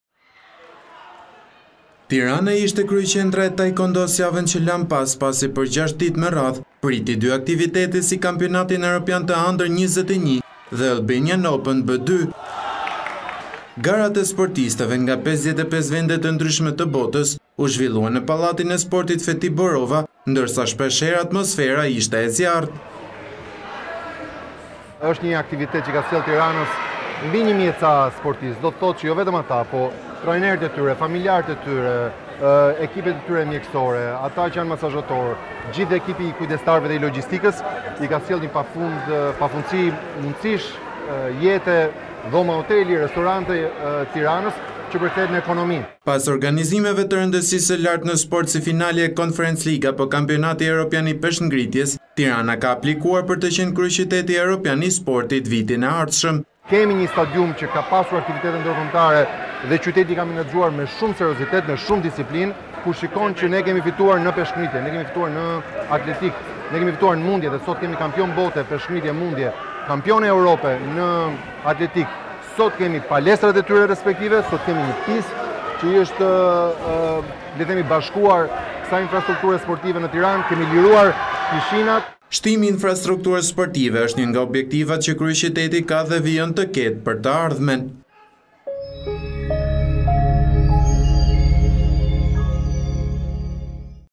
Tirana pret Evropianin e Taekwondo, Veliaj: Gati për të qenë Kryeqyteti Europian i Sportit
reportTV lajmeshqip lajme Për gjashtë ditë me radhë Tirana priti Kampionatin Europian të Taekëondo's Under 21 dhe Albanian Open B2, duke bërë bashkë mbi 1000 sportistë nga 35 shtete të Europës dhe nga 55 shtete të botës.